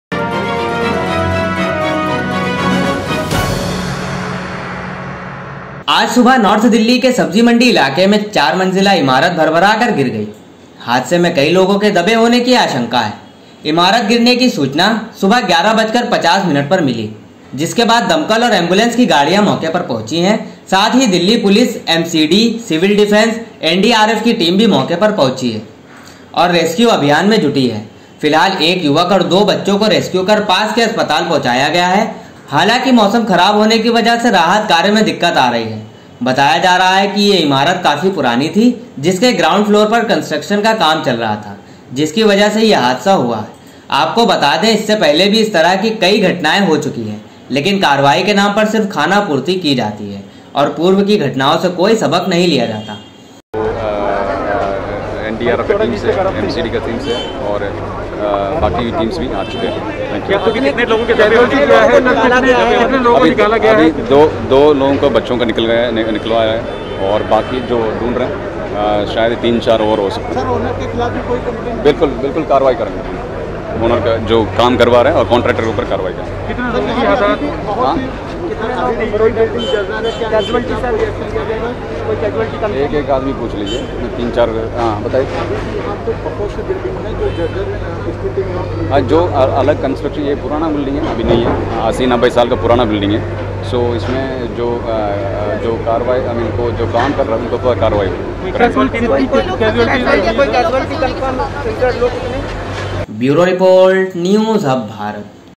North Delhi के सब्जी मंडी इलाके में 4 मंजिला इमारत गिरने से कई लोग दबे, इलाके में दहशत | News Hub
About this video-आज सुबह नॉर्थ दिल्ली के सब्जी मंडी इलाके में एक 4 मंजिला इमारत भर भरा कर गिर गई, हादसे में कई लोगों के दबे होने की आशंका है,इमारत गिरने की  सूचना 11:50 पर मिली थी,जिसके बाद दमकल और एंबुलेंस की  गाड़ियां मौके पर पहुंची हैं,साथ ही दिल्ली पुलिस, एमसीडी ,सिविल डिफेंस, एनडीआरएफ की टीम भी मौके पर पहुंची हुई है और रेस्क्यू अभियान मै जुटी हुई है,फिलहाल 1 युवक और 2 बच्चो को रेस्क्यू कर पास के अस्पताल पहौचाया गया है,हालाकि मौसम खराब होने की वजह से राहत कार्य मै दिक्कत आ रही है,बताया  जा रहा है, कि यह इमारत काफी पुरानी थी,जिसके  ग्राउंड फ्लोर पर कंस्ट्रक्शन का काम चल रहा था,जिसकी वजह से ये हादसा हुआ,आप को बतला दे इससे पहले भी इस तरह की कई घटनाएं हो चुकी है लेकिन कार्यवाही के नाम पर सिर्फ खाना पूर्ति की जाती है,ओर पूर्व की घटनाओं से कोई सबक नहीं लिया जाता है,फिलहाल पीड़ित परिवार अपनो की तलाश मै मौके पर पहुंचे हुए है,jओर अपनो की सलामती किं दुआए मांग रहे है
जो तस्वीरें आप देख रहे हैं यह तस्वीरें दिल्ली के सब्जी मंडी इलाके की उसी जगह की है  जहां पर  इमारत गिरी है हादसे के बाद स्थानीय लोग मौके पर एकत्रित हुए हैं और बचाव कार्य अपने स्तर पर चलाना शुरू किया हालांकि सूचना के बाद मौके पर दमकल और स्थानीय पुलिस एजेंसी ऑफ पहुंचकर बचाव कार्य में जुट गई हैं  हालांकि अभी तक अधिकारिक तौर पर हताहतों के संख्या का पता नहीं चल पाया है।